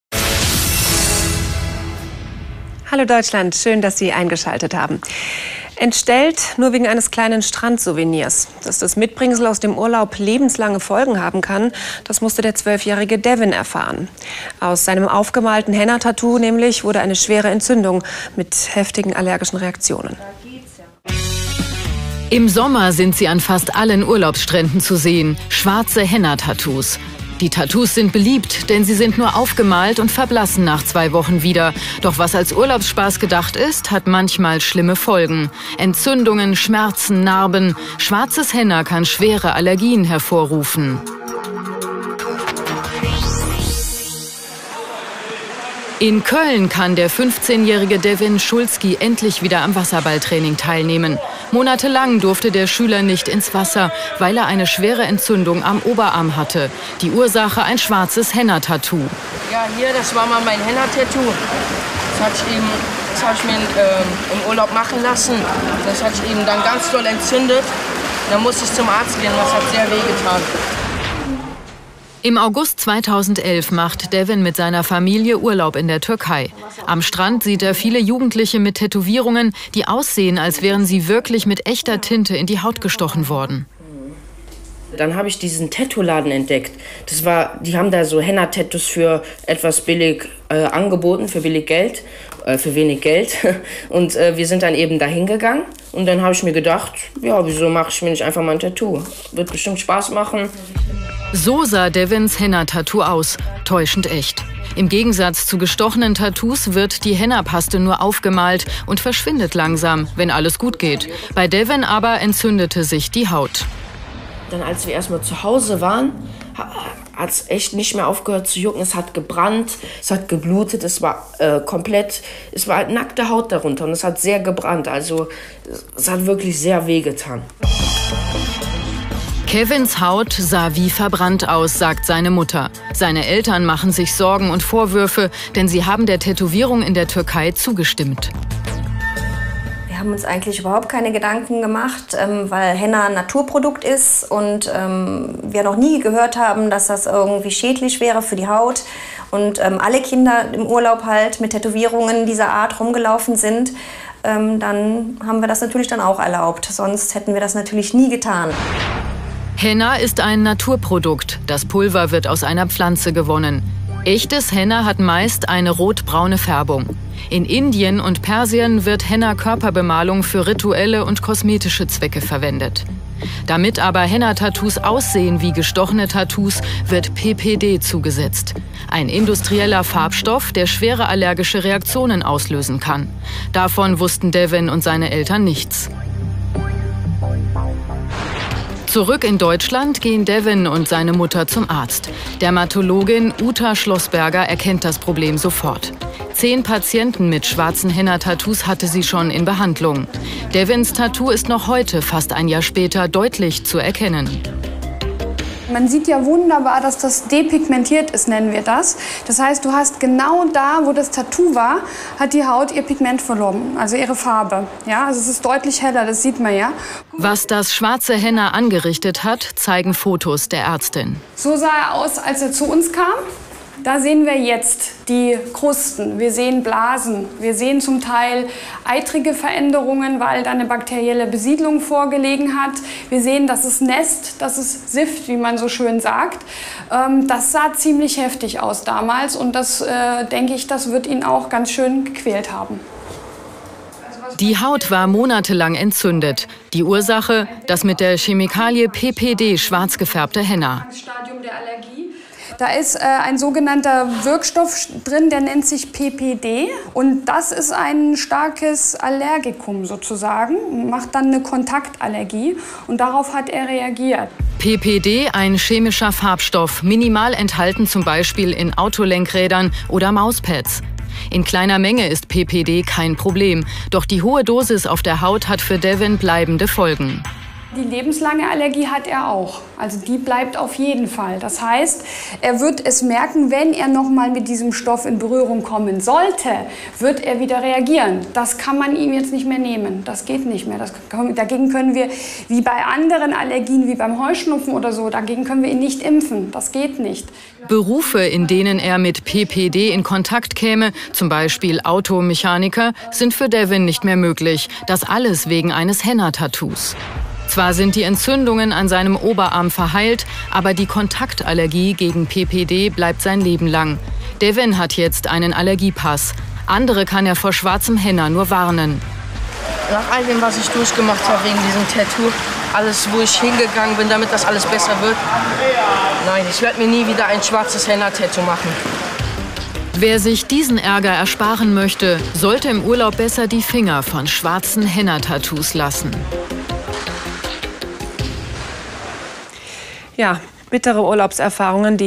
(schwarze) Henna- Tattoos .
(schwarze) Henna Tattoos. Nebenwirkungen. Was kann passieren?